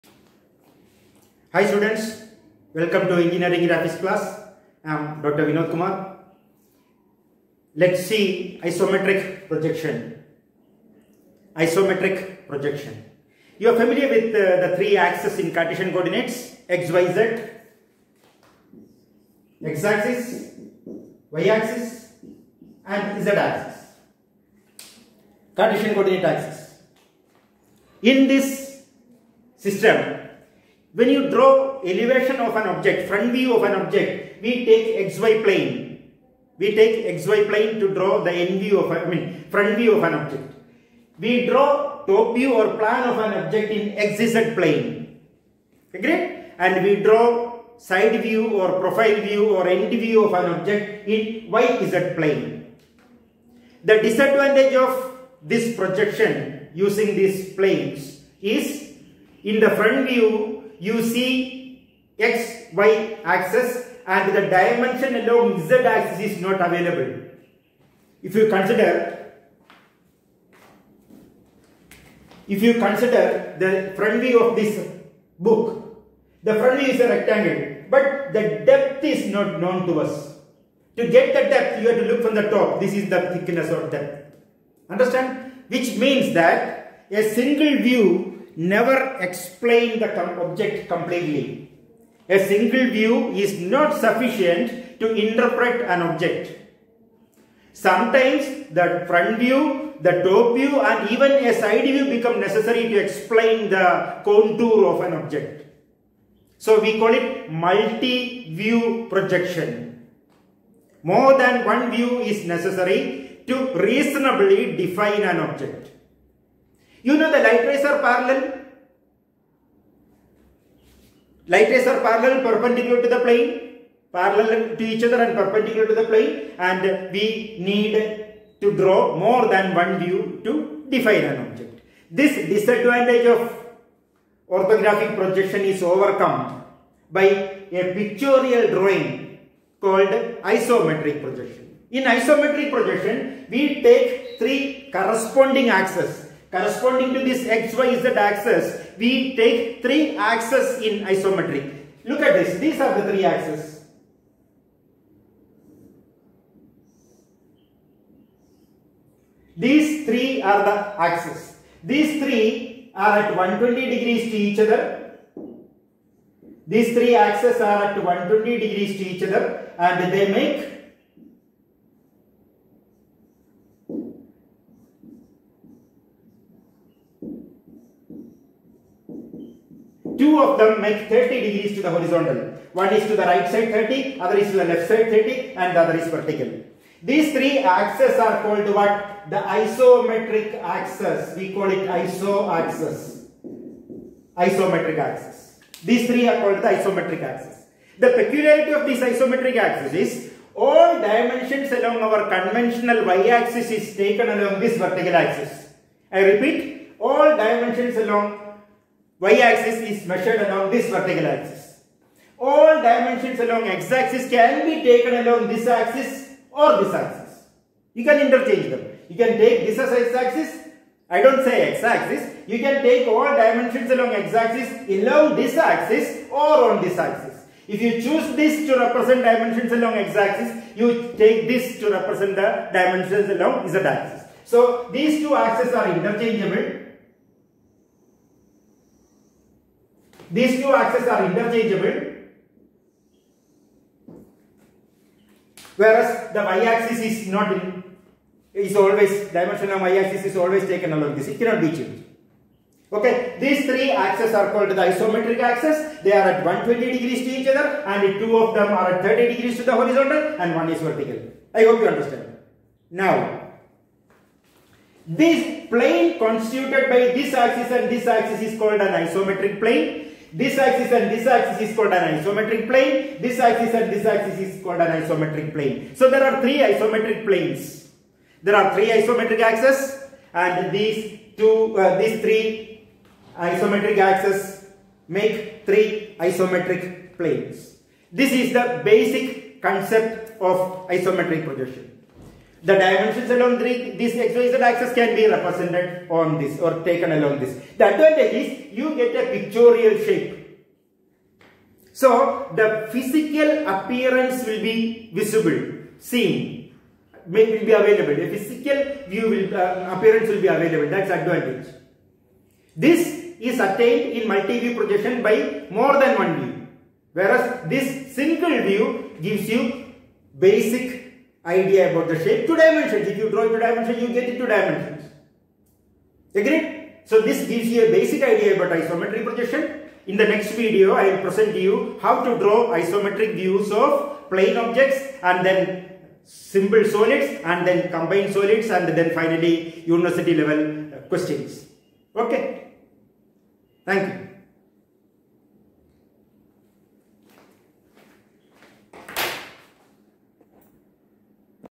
ISOMETRIC PROJECTION -1, INTRODUCTION by Prof. Vinodkumar
Introduction to isometric projection, isometric axes, isometric planes etc.